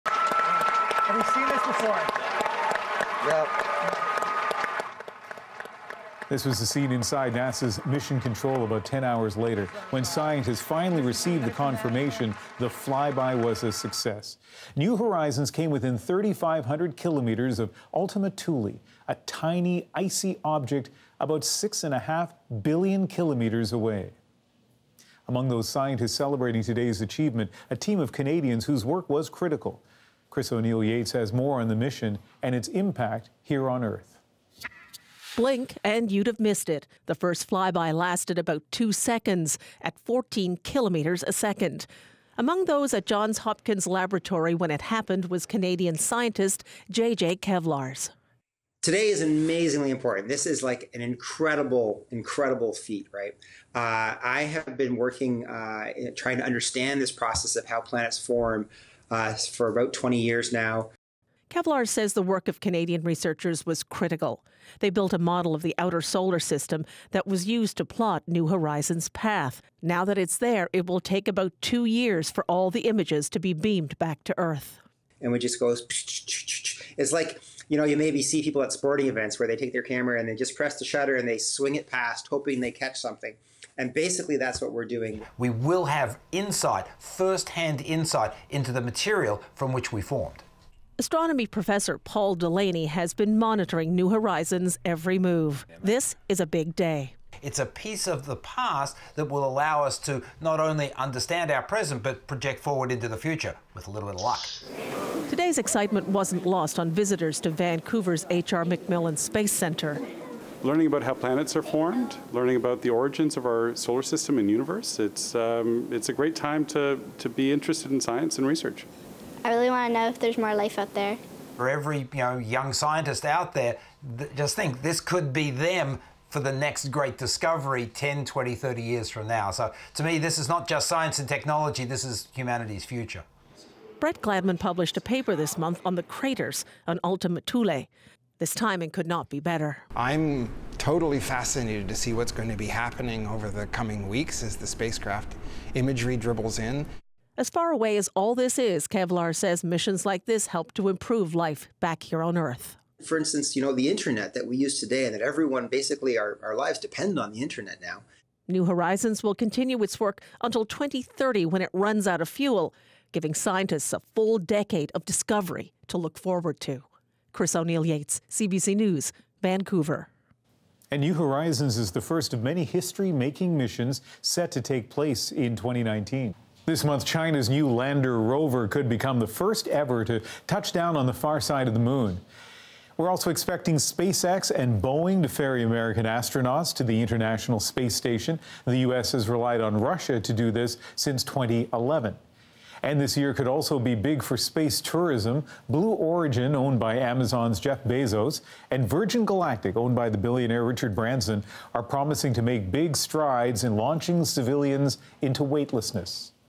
NASA's New Horizons spacecraft completes historic flyby at edge of solar system
NASA's New Horizons spacecraft made a successful flyby of the tiny, icy celestial object known as Ultima Thule, 6.5 billion kilometres from Earth, and the work of Canadian researchers was critical to the historic mission.